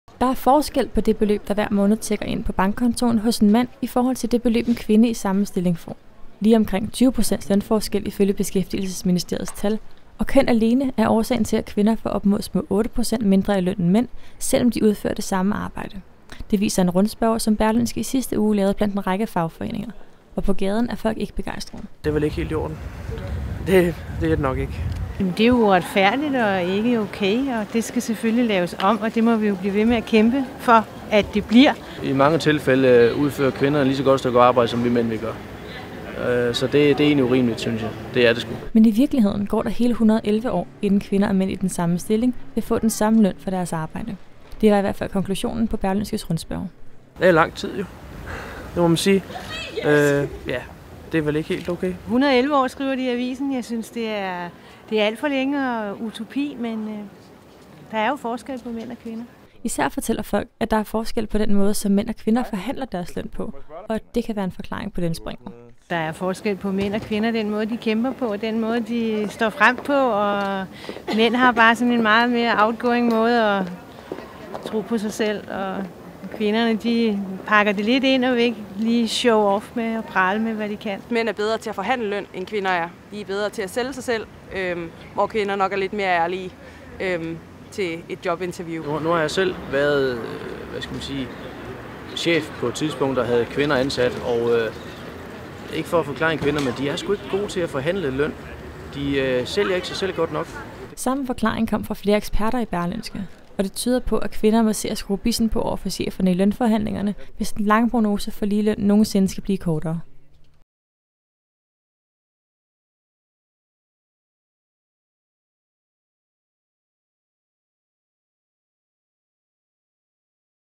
TV: Lang udsigt til ligeløn overrasker danskerne
Af Line Jenvall og Kristoffer Klit Langgard

Der er som bekendt forskel på mænd og kvinder. Mere overraskende er det måske, at der også er forskel på den betaling, mænd og kvinder får for det samme stykke arbejde, og at udsigten til, at den forskel forsvinder, er lang.

Men ifølge en rundspørge, som Berlingske i sidste uge lavede blandt en række fagforeninger, skyldes op mod små otte procent af lønforskellen mellem mænd og kvinder alene køn.

Og vi skal et godt stykke ind i det næste århundrede, før mænd og kvinder i de samme stillinger også får det samme i løn, hvis udviklingen på området fortsætter i samme sløve tempo som indtil nu.

Slet ikke i orden, lyder reaktionen fra danskerne.

- I mange tilfælde udfører kvinder et lige så godt stykke arbejde, som vi mænd gør. Så det er egentlig urimeligt, siger Michael Nørgaard.

Især det faktum, at der ifølge prognosen vil gå 111 år, inden lønudbetalingerne for mænd og kvinder i samme stillinger bliver ens, forbavser danskerne.

- Jeg synes det er alt for længe og utopi, fortæller Jette Heisselberg.

Kvinderne skal frem i skoene

Men danskerne erkender, at der er forskel på mænd og kvinder, når det gælder indstillingen til lønforhandling, og vurderer, at man nok kan finde del af forklaringen på lønspringet netop ved chefens forhandlingsbord.

- Mænd er bedre til at forhandle løn end kvinder er. De er bedre til at sælge sig selv, hvorimod kvinder nok er mere ærlige til et jobinterview for eksempel, forklarer Christina Thomsen.

Kvinderne skal med andre ord lære at træde i karakter og kræve, hvad de fortjener på samme måde, som mændene gør.

- Vi mænd er nok hårdere overfor vores chef, når vi siger: ”Jeg er fandeme det værd!” Der skal kvinder lære at sige: ”Jeg kan det her!”, siger Michael Nørgaard.

Og hvis kvinderne begynder at skrue bissen på i lønforhandlingerne, kan det være, de 111 år bliver til færre i fremtiden.